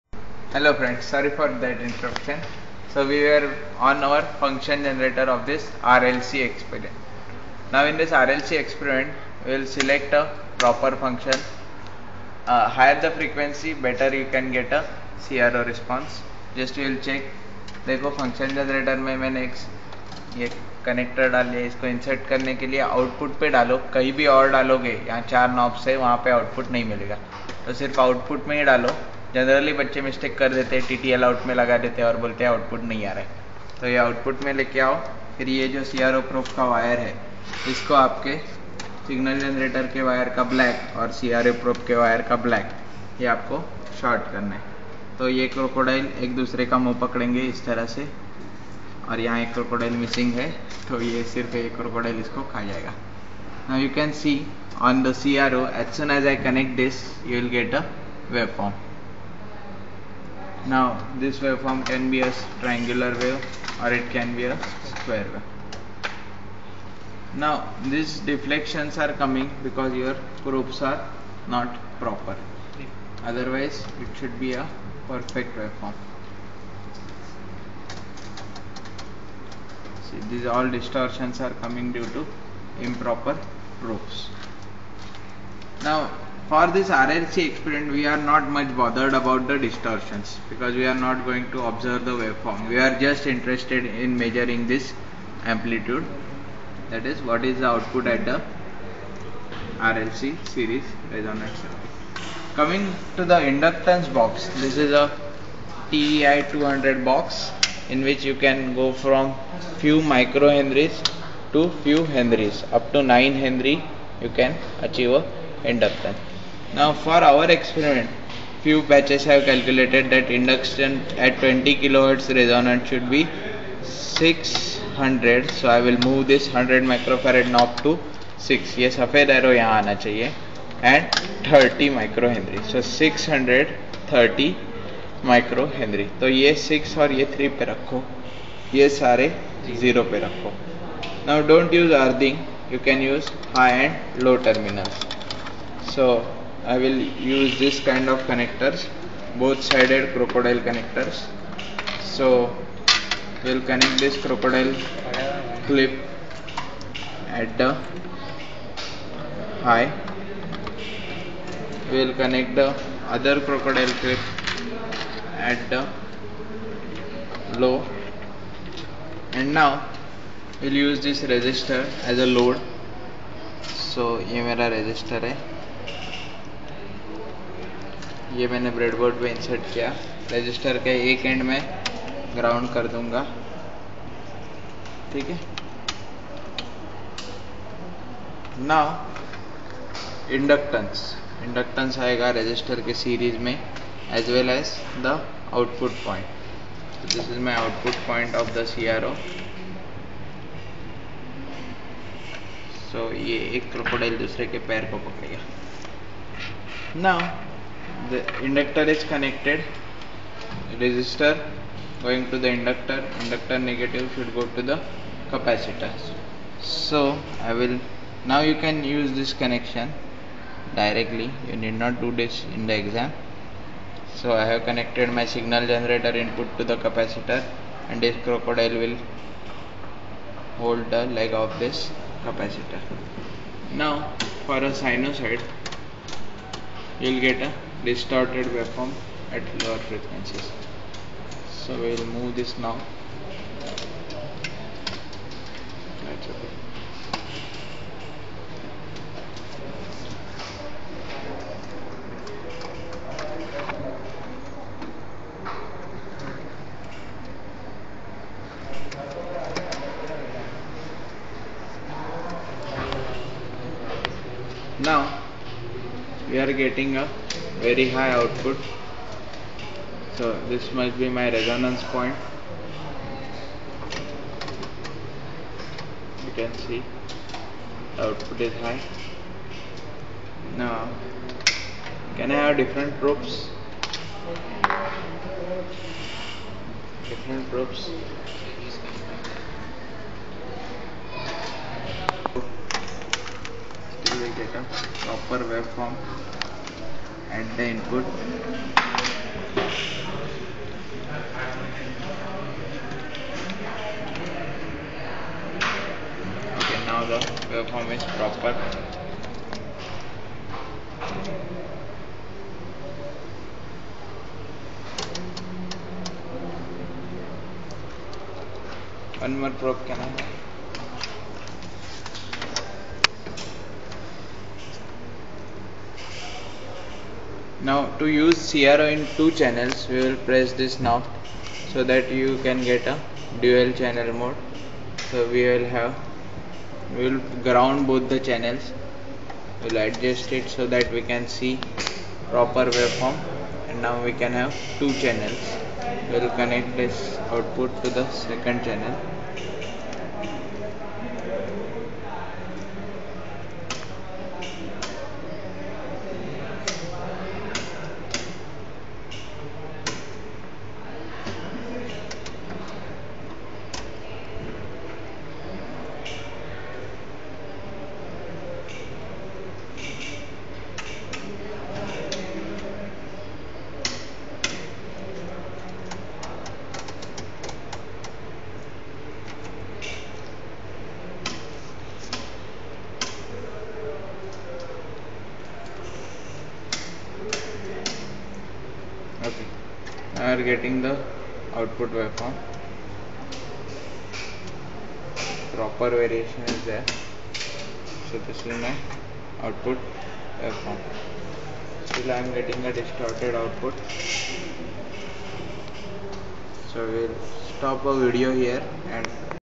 RLC2 OF 3
This video is to help my 1st year students in how to perform the RLC experiment
"best of luck students !" : Prof.Ninad Mehendale